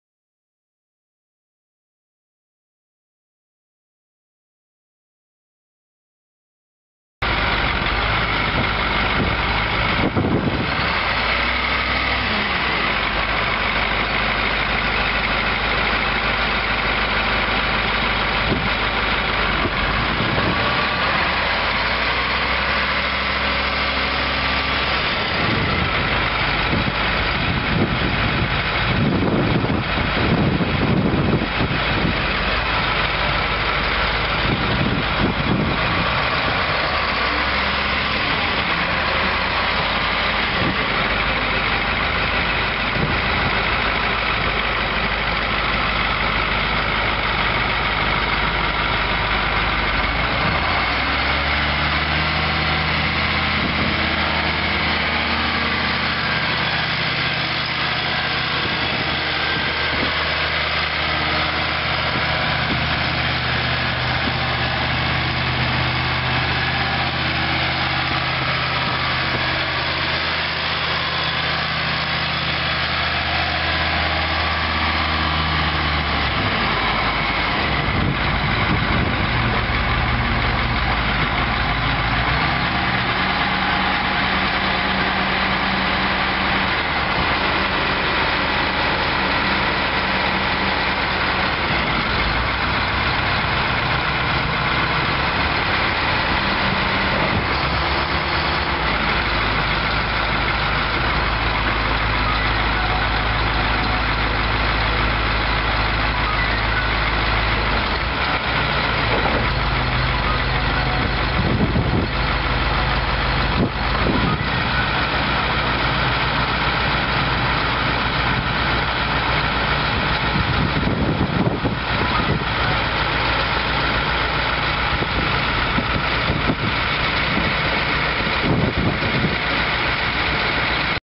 Caterpillar V160 forklift for sale | sold at auction October 8, 2015
Caterpillar V160 forklift
533 hours on meter
Actual hours unknown
Caterpillar 3208 eight cylinder diesel engine
Serial 32Y8309
Manual transmission
OROPS
16,000 lbs maximum lift capacity
218" maximum lift height
Two mast stages
Side shift
Mast tilt
6&#039; fork length
67"W carriage
9.00-20 NHS dual front tires
9.00-20 NHS rear tires
Weld repairs to carriage
Serial 16V213